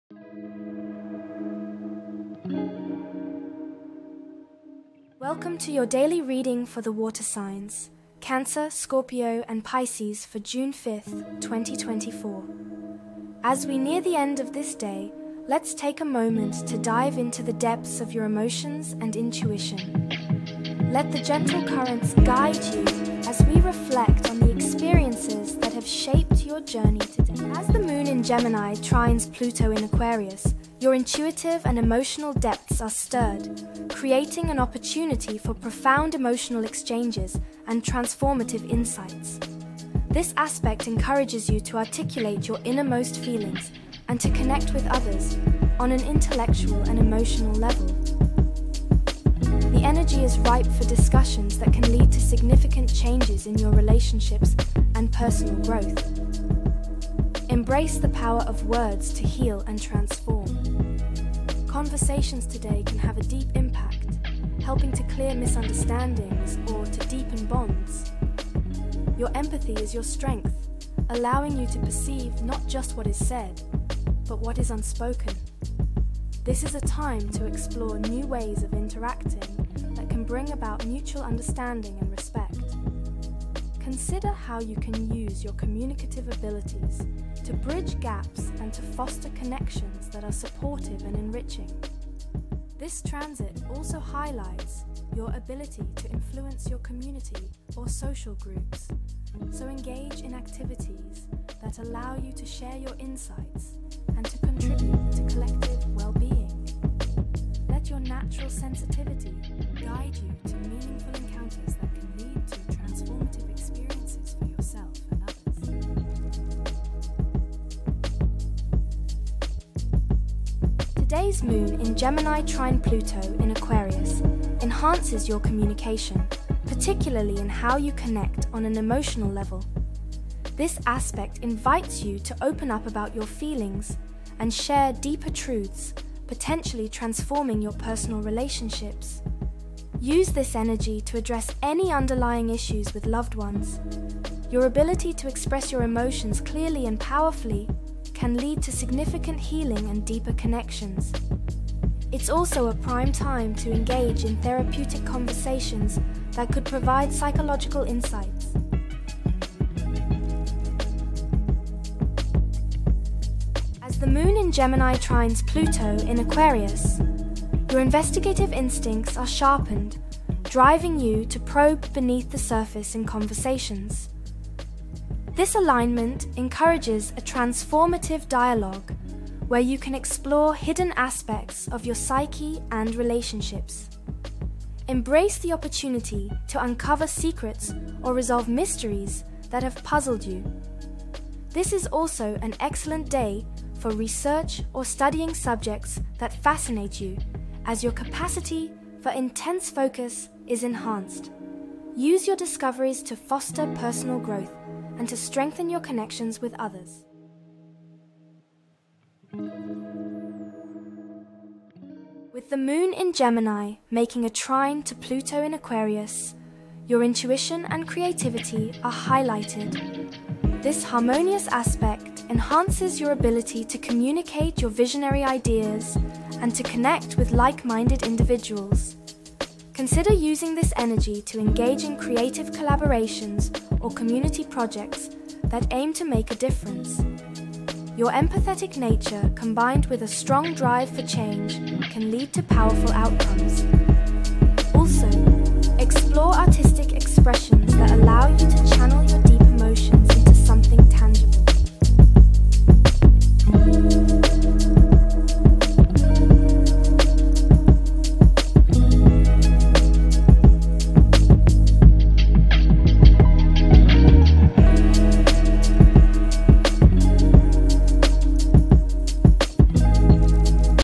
Water Signs Overview for June 5, 2024
As the Moon in Gemini trines Pluto in Aquarius, your intuitive and emotional depths are stirred, creating an opportunity for profound emotional exchanges and transformative insights. This aspect encourages you to articulate your innermost feelings and to connect with others on an intellectual and emotional level. The energy is ripe for discussions that can lead to significant changes in your relationships and personal growth. Embrace the power of words to heal and transform; conversations today can have a deep impact, helping to clear misunderstandings or to deepen bonds. Your empathy is your strength, allowing you to perceive not just what is said, but what is unspoken. This is a time to explore new ways of interacting that can bring about mutual understanding and respect. Consider how you can use your communicative abilities to bridge gaps and to foster connections that are supportive and enriching. This transit also highlights your ability to influence your community or social groups, so engage in activities that allow you to share your insights and to contribute to collective well-being. Let your natural sensitivity guide you to meaningful encounters that can lead to transformative experiences for yourself and others.